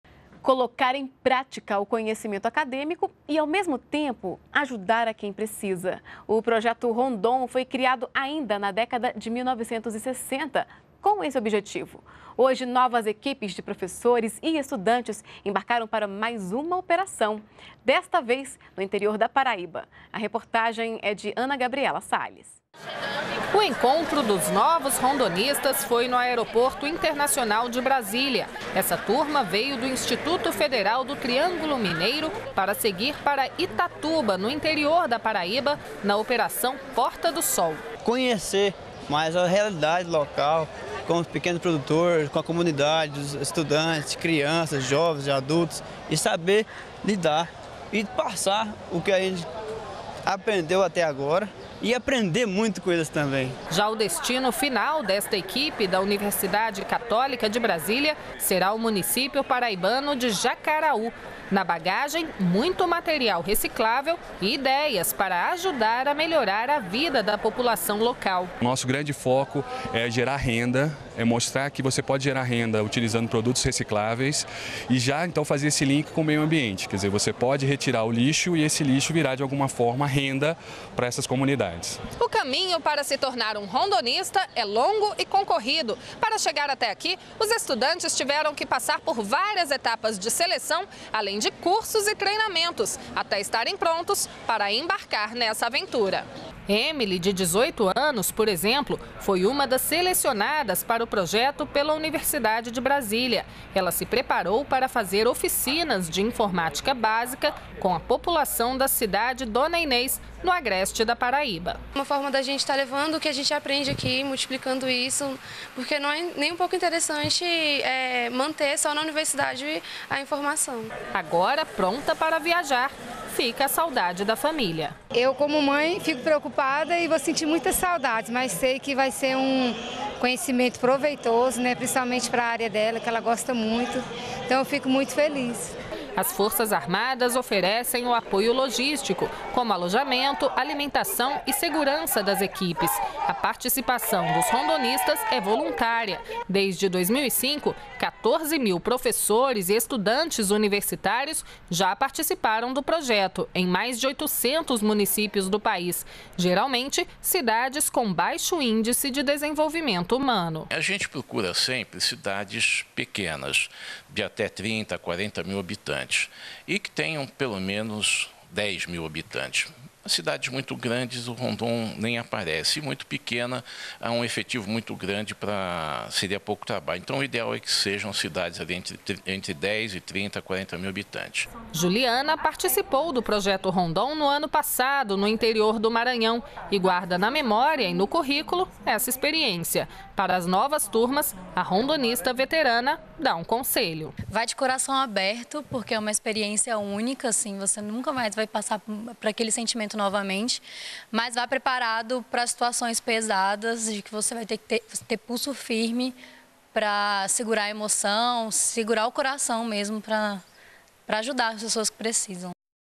Projeto Rondon estimula intercâmbio de professores e estudantes com municípios com baixo IDH
NBR NOTÍCIAS - 22.01.15: As atividades do Projeto Rondon em 2015 já começaram. Entre as operações programadas para este ano, está a Porta do Sol, que será realizada de 24 de janeiro a 7 de fevereiro, na Paraíba. O Projeto Rondon, coordenado pelo Ministério da Defesa, tem o objetivo de estimular a participação de estudantes universitários no processo de desenvolvimento sustentável e fortalecimento da cidadania em municípios isolados e com baixo Índice de Desenvolvimento Humano (IDH). Desde 2005, a iniciativa atendeu a cerca de 830 municípios, contando com a participação de quase 14 mil professores e estudantes universitários.